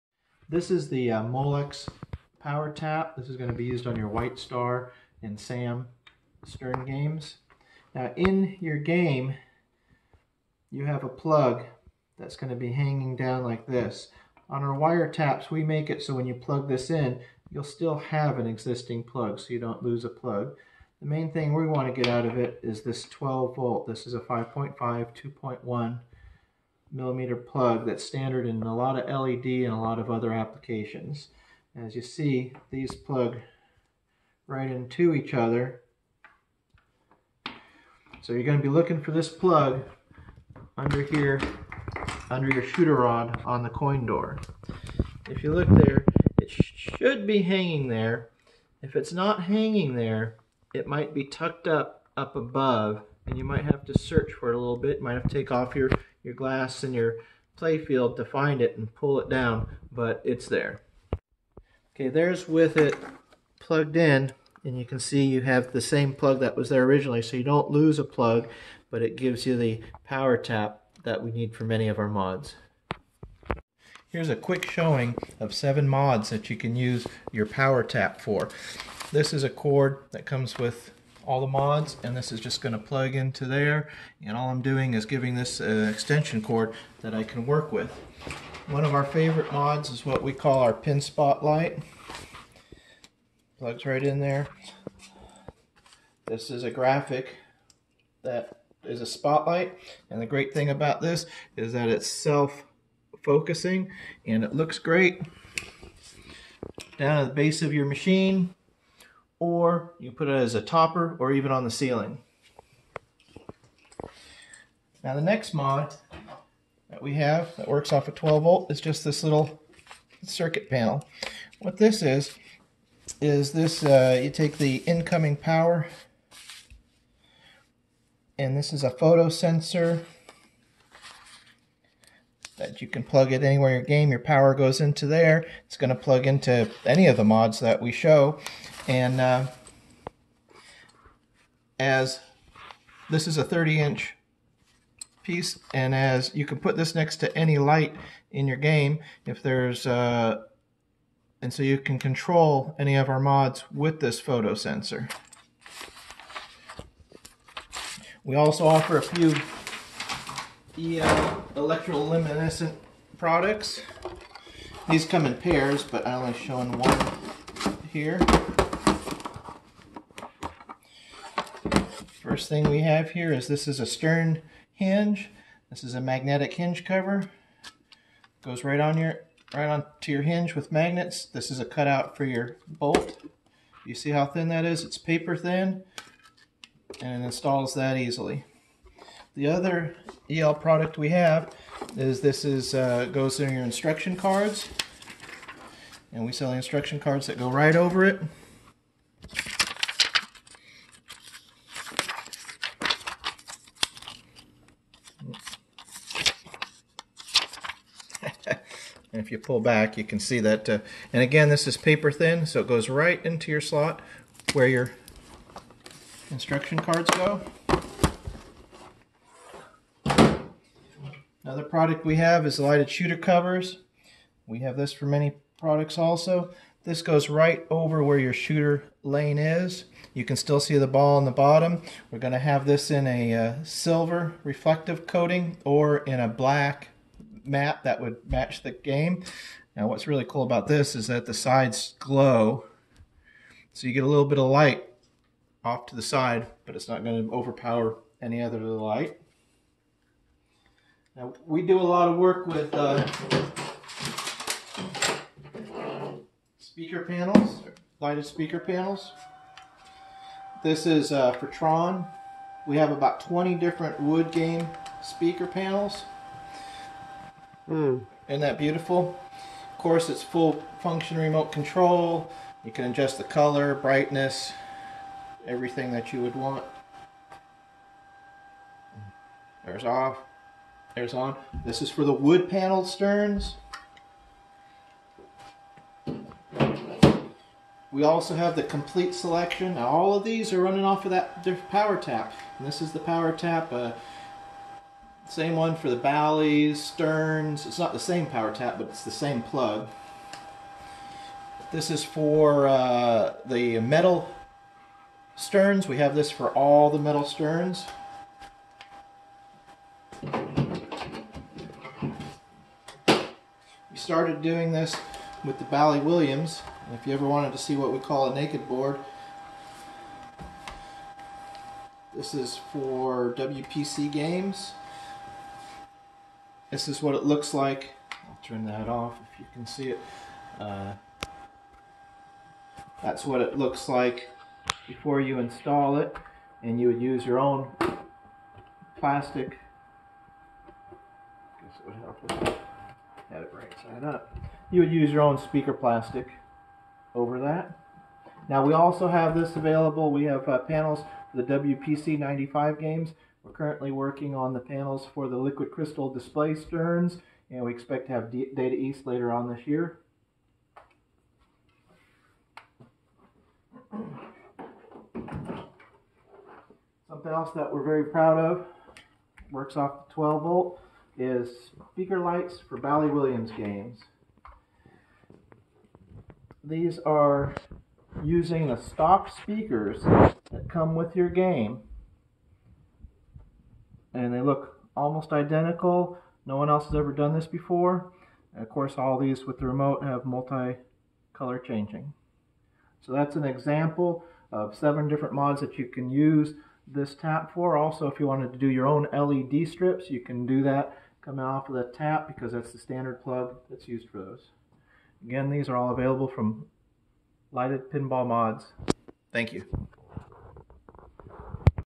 Stern power tap for SAM and Whitestar molex pinball machines with mods
This video demonstrates how to install a power tap in a Stern Pinball Machine using a Molex plug.  We sell these at lightedpinballmods.com and these are included in many of our pinball machine mods that run off of 12V. The mods at the end of the video are just a quick example of mods you can power with the tap.  We have better videos of these products under the sales page of those items.